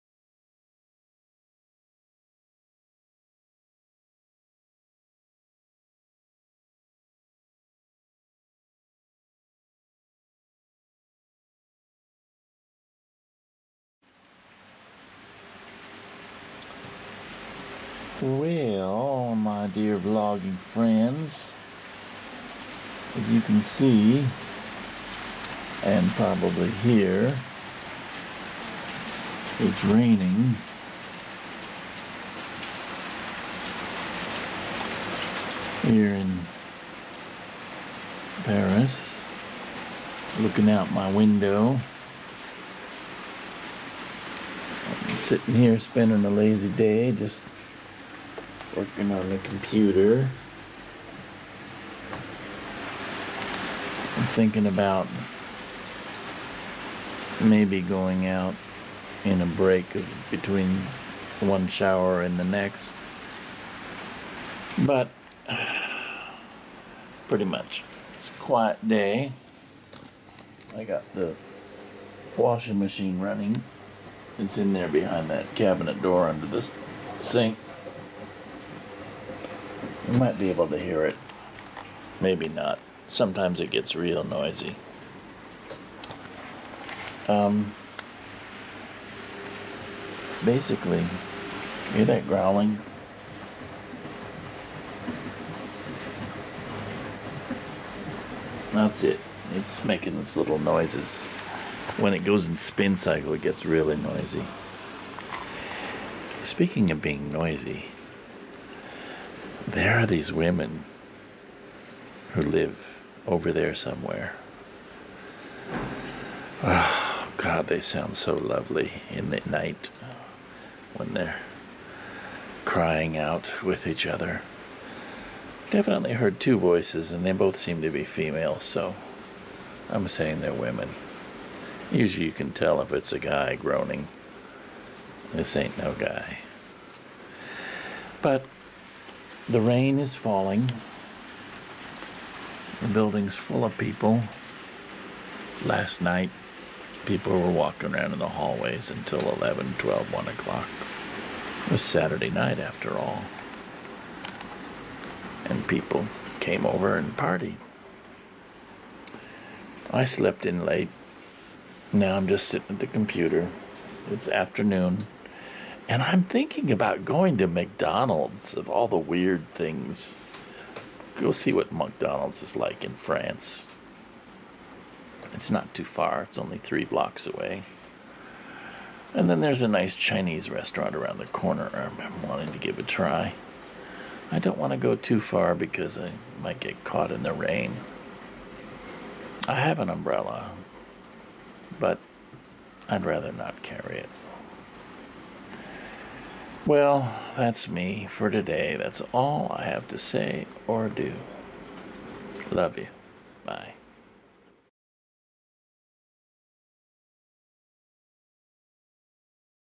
vlogging around the Louvre and Paris #35 -- at home with the rain
Edited from video recorded 12 October 2014. At home in my little rented room with my laptop in the rain. Doing laundry and looking out the window and thinking about things.